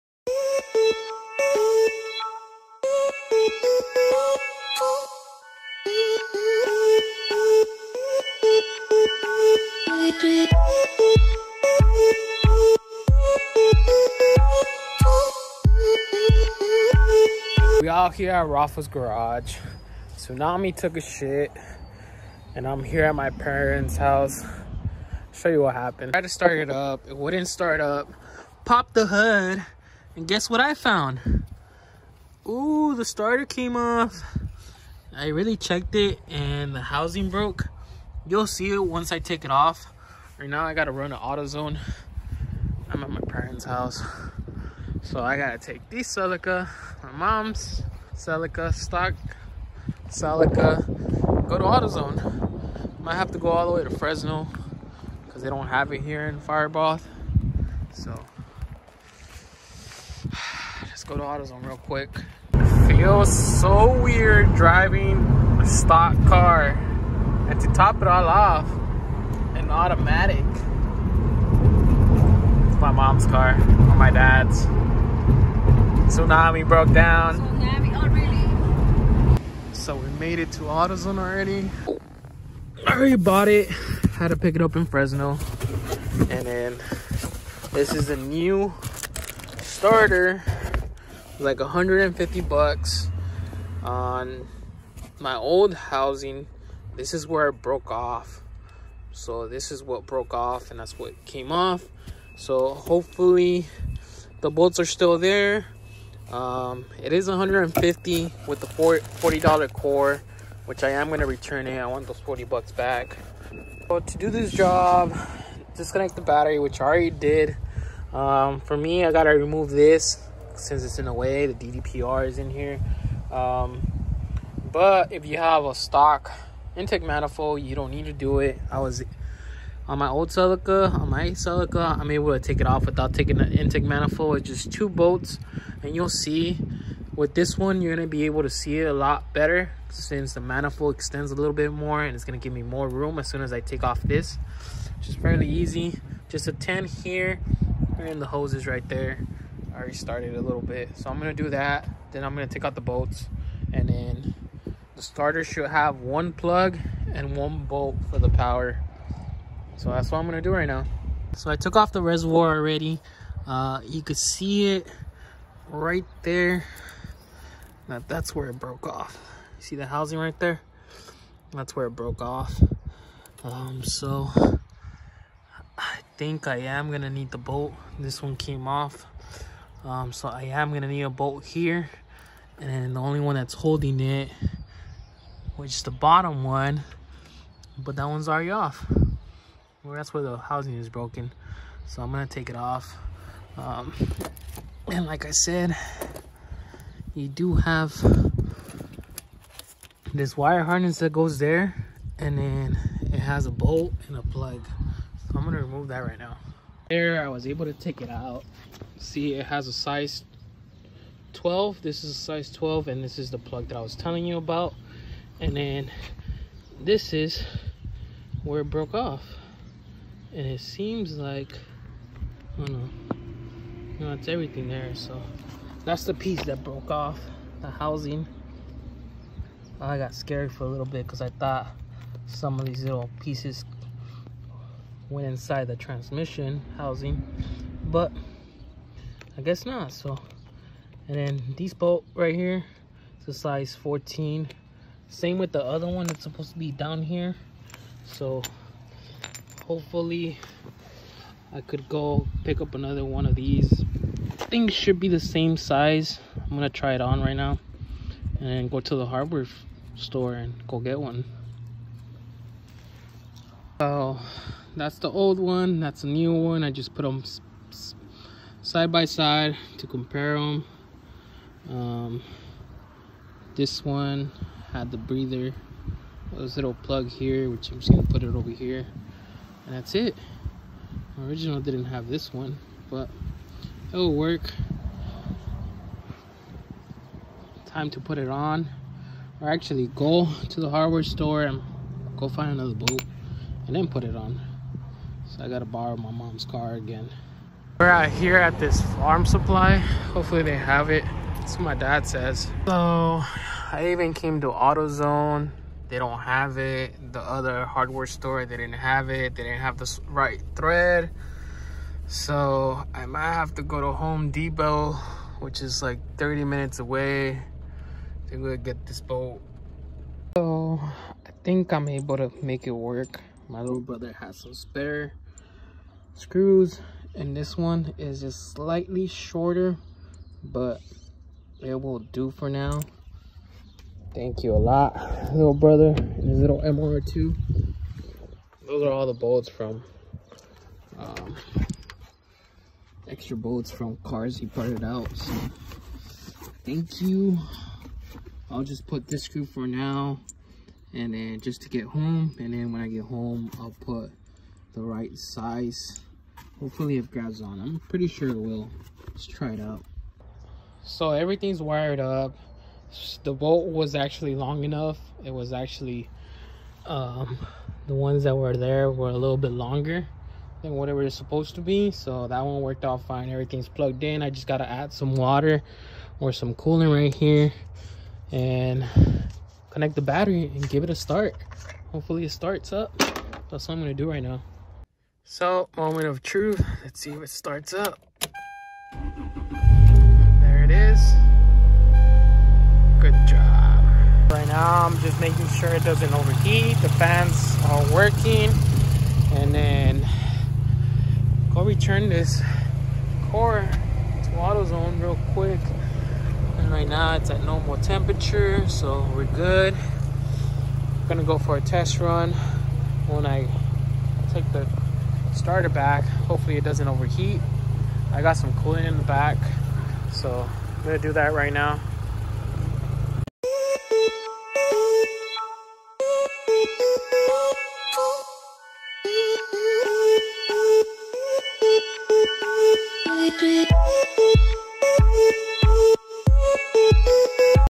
Tsunami Starter Broke!!!
Today when trying to start up the car wouldn't crank over so pop the hood and I find out the housing on the starter broke. So had to go to autozone in Fresno and replace it in the cold.